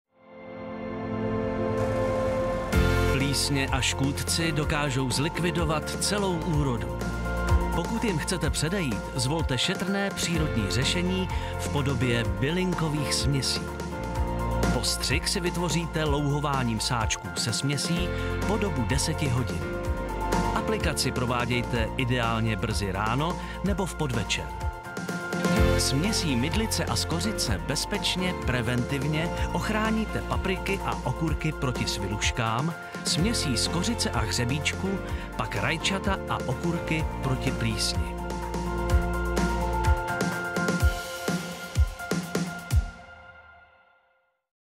MINUTOVKA - Přírodní ochrana rostlin (NATURA Bylinkové směsi)
Rychlý návod, jak rostliny preventivně chránit před plísněmi a sviluškami - navíc přírodně!